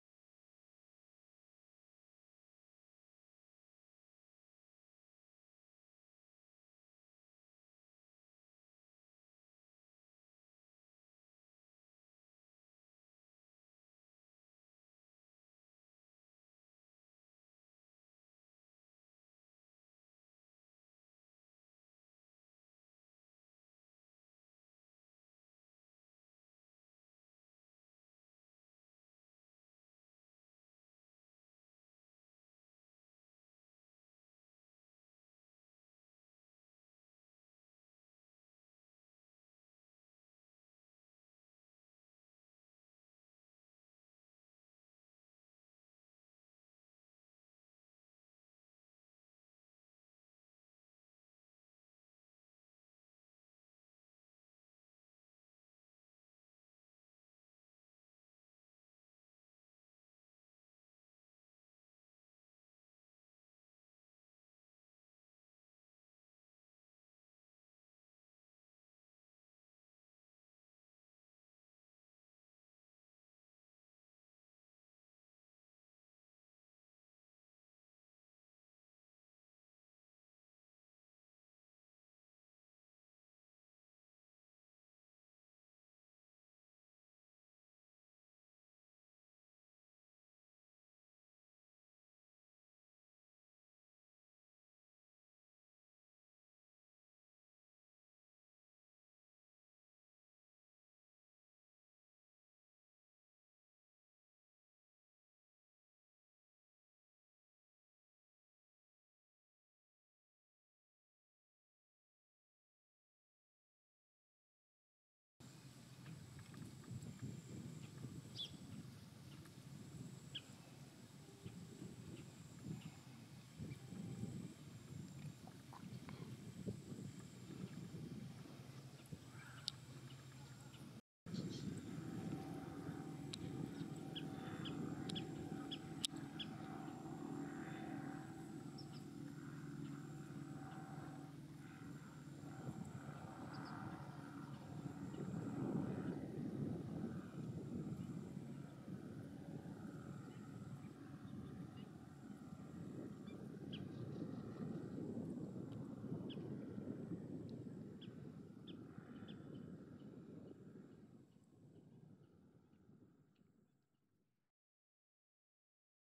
Cannaiole - Reed warbler (Acrocephalus scirpaceus)
IT: Cannaiola (Acrocephalus scirpaceus), è difficile filmare le Cannaiole e ancora più difficile é filmarne due o più insieme.
EN: Eurasian Reed warbler (Acrocephalus scirpaceus), it is difficult to film the Reed warblers, and even more difficult is to film two or more of them together.